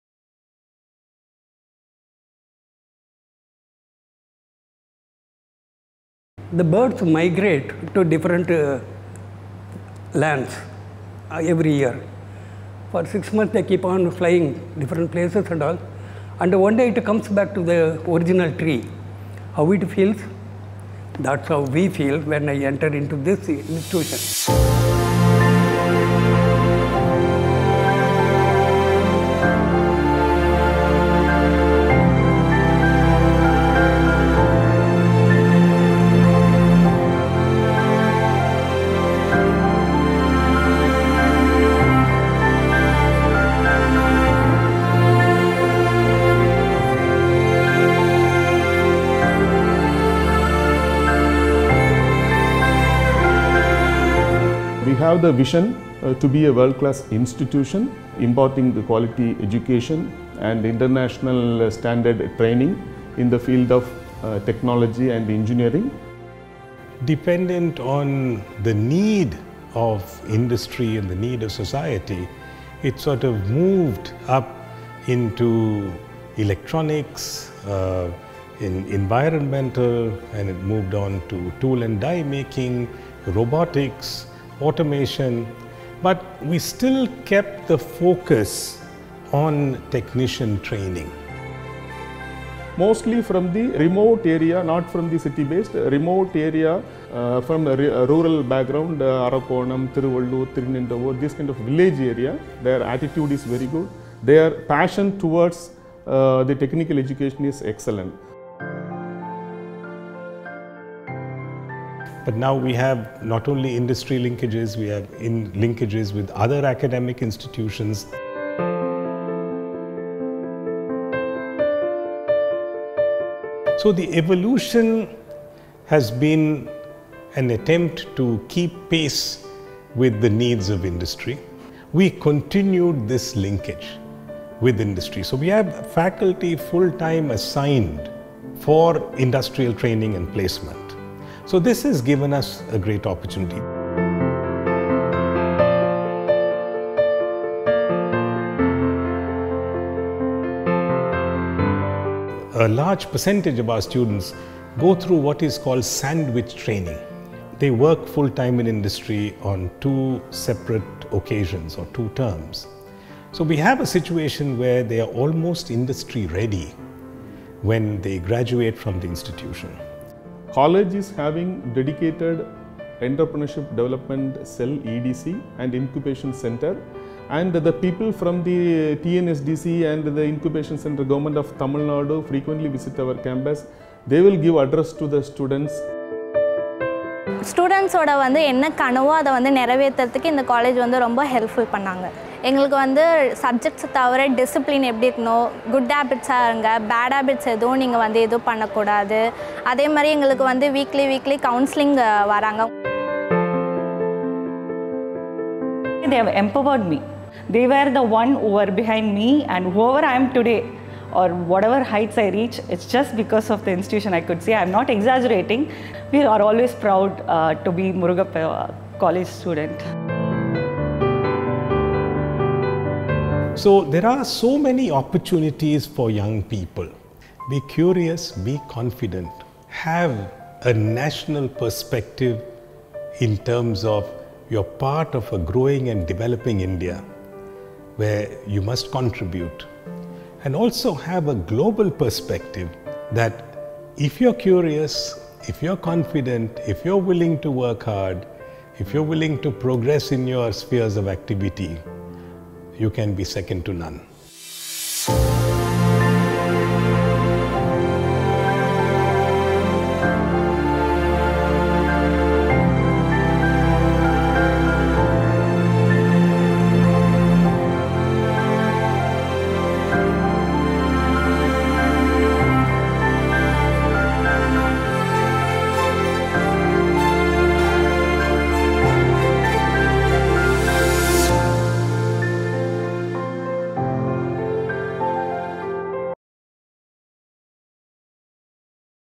Murugappa Polytechnic College (MPC) | AMM Foundation | A Century of Service
Imparting quality education in the fields of Engineering, Technology and beyond, this is the story of the Murugappa Polytechnic College (MPC).